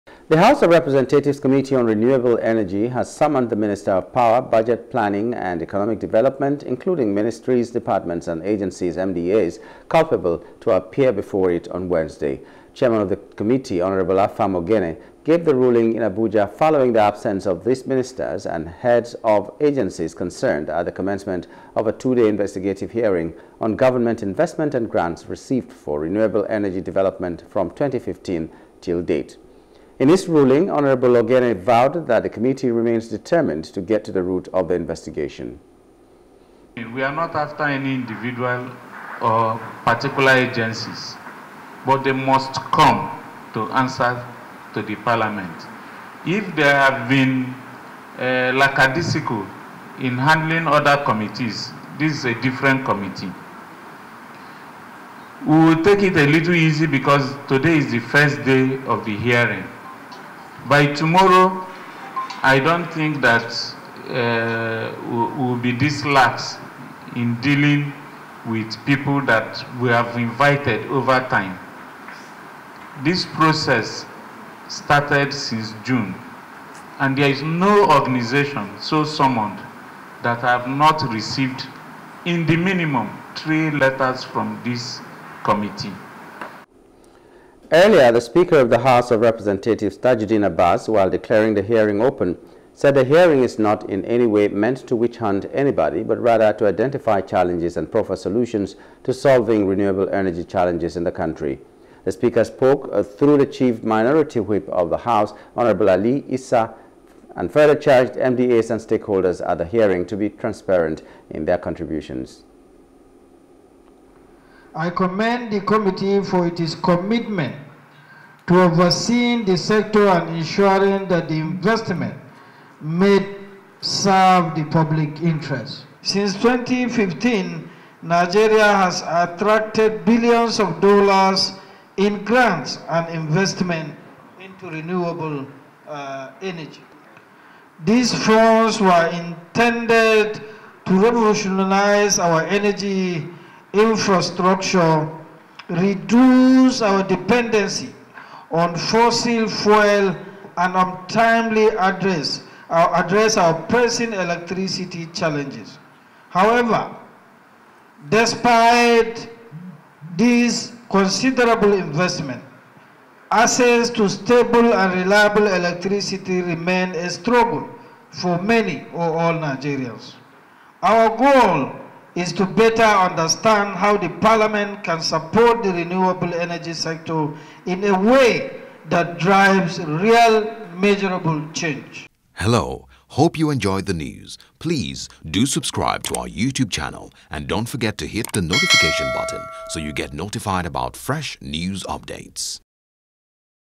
Renewable Energy Reps Summon Power Minister, Mdas Over Unutilized Grants
The House of Representatives Committee on Renewable Energy has summoned the Ministers of Power, Budget Planning and Economic Development, including Ministries Departments and Agencies culpable to appear before it on Wednesday.

Chairman of the Committee, Hon. Afam Ogene, gave the ruling in Abuja, following the absence of these ministers and heads of agencies concerned at the commencement of a two-day investigative hearing on government investments and grants received for renewable energy development from 2015 till date.

In his ruling, Hon. Ogene vowed that the committee remains determined to get to the root of the investigation.

Earlier, the Speaker of the House of Representatives, Tajudeen Abbas, while declaring the hearing open, said the hearing is not in any way meant to witch-hunt anybody but rather to identify challenges and proffer solutions to solving renewable energy challenges in the country.

The speaker spoke through the Chief minority whip of the house, Hon. Ali Isah further charged MDAs and stakeholders at the hearing to be transparent in their contributions.